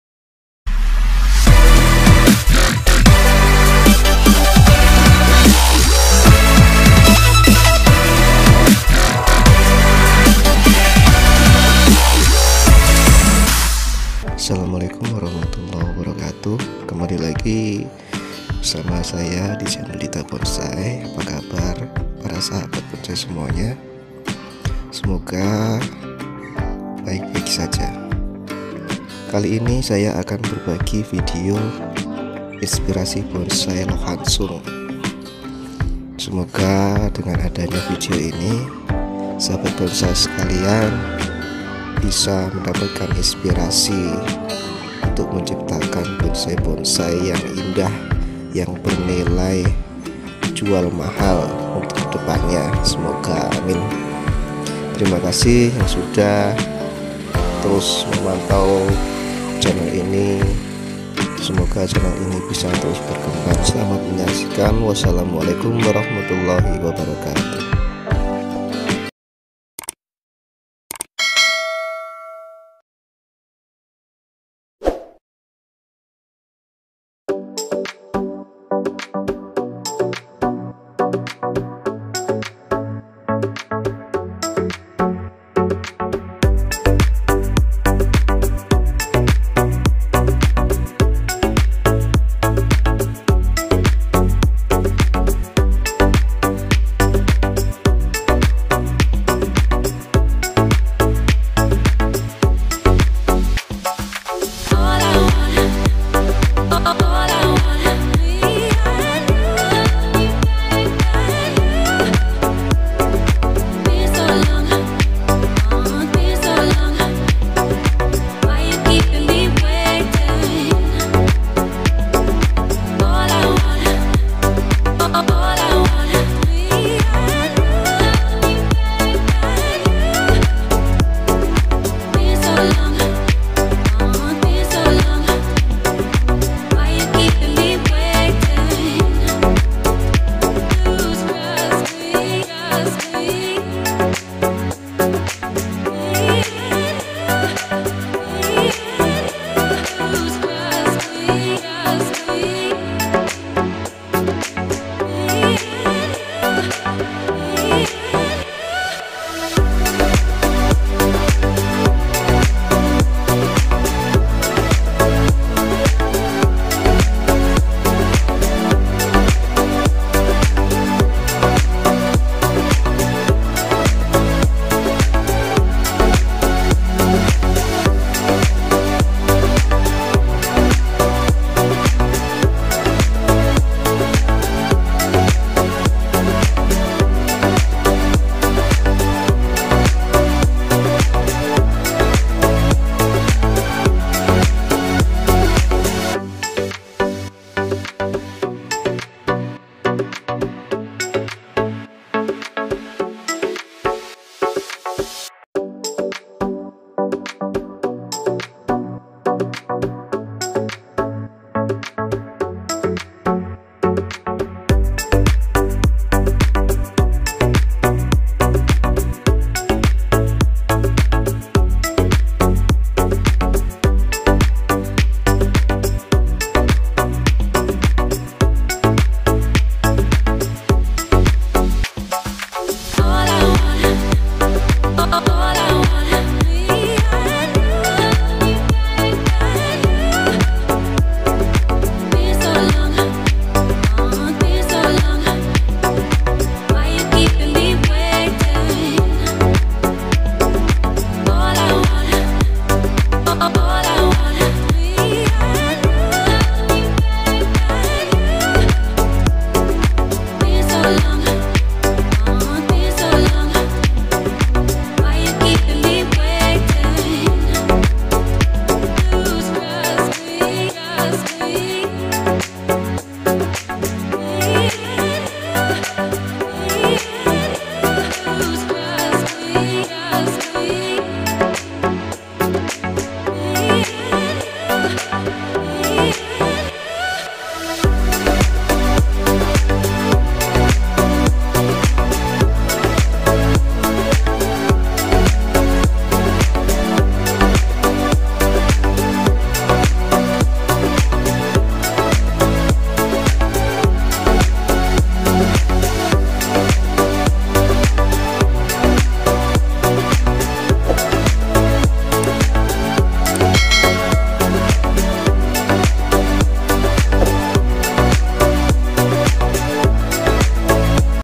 Bonsai lohansung terbaik 2022 inspirasi | The best Lohansung bonsai for 2022 inspiration
Lohansung (Podocarpus macrophyllus) dikenal sebagai Kusamaki atau Inumaki, merupakan tanaman conifer dari genus Podocarpus, famili Podocarpaceae. Lohansung berasal dari Jepang dan juga China timur ke selatan. Kusamaki dan Inumaki adalah Bahasa Jepang untuk pohon lohansung, namun Kusamaki belakangan ini sudah digunakan oleh orang Amerika dan Eropa untuk menyebut tanaman ini, menggantikan nama dulunya yaitu Buddhist Pine atau Fern Pine (meski sebenarnya bukan keluarga pine atau pinus sama sekali), nama lain yang juga lazim disebut adalah chinese yewr.

Di China tanaman ini dinamai luohan song, yang kemudian menjadi asal dari nama tanaman ini di Indonesia, yaitu lohansung. Luohan song dalam Bahasa China berarti arhat pine (arhat dalam Buddhism berarti orang yang telah tercerahkan). Lohansung sangatlah populer di Jepang, bahkan dijadikan pohon resmi untuk bagian wilayah Chiba Prefecture di Jepang. Buahnya dapat dimakan, namun bijinya tidak bisa.

Batang dari lohansung sangat tahan terhadap rayap dan juga air, sehingga digunakan sebagai bahan dasar pembuatan rumah kayu berkualitas di Okinawa Prefecture di Jepang. Tanaman ini juga sangat tinggi harganya di Hong Kong karena bernilai feng shui sangat bagus. Bahkan dalam beberapa tahun terakhir, penebangan ilegal terhadap lohansung di Hongkong merupakan masalah bagi kota ternama di dunia tersebut.

Di Indonesia lohansung banyak dicari sebagai subjek bonsai, cantik dengan daunnya yang kecil.